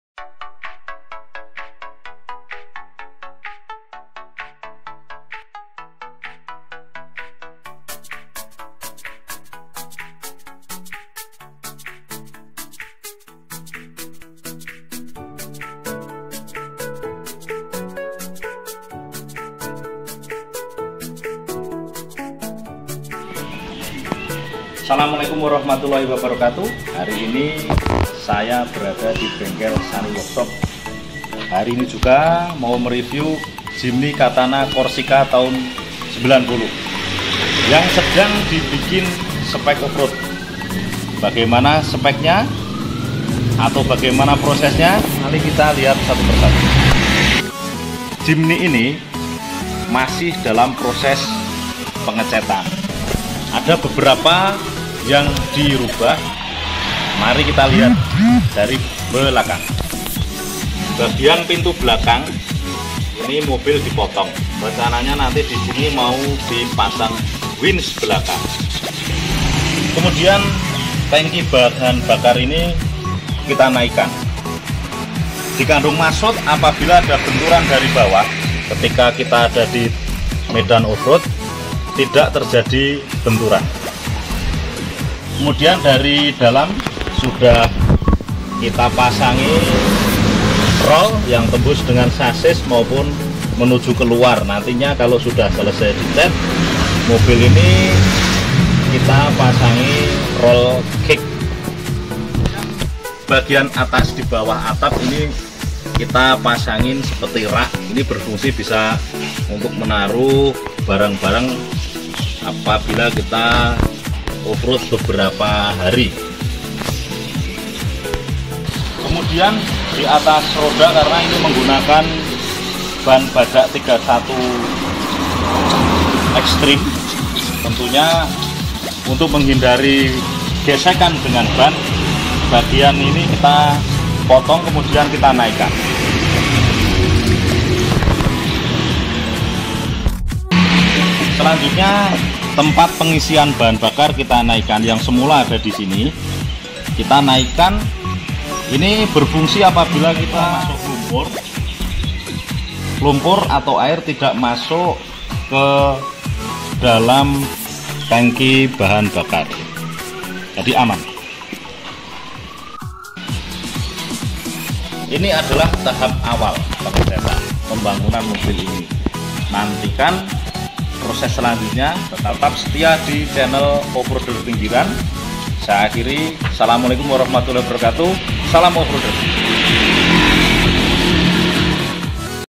Proses Modifikasi Jimny katana Corsica 1990 di buat Jimny speck Offroad siap tempur di Trex extream
Proses dalam rangka pengecetan, namun sebelumnya pintu belakang d potong untuk tempat winch, tanki bbm d naikan, tempat pengisian juga d naikan, pemasangan rak dalam dan juga roolcage, dan juga pemotongan fender agar tdk terjadi gesekan dengan ban.
hanya di Sani workshop.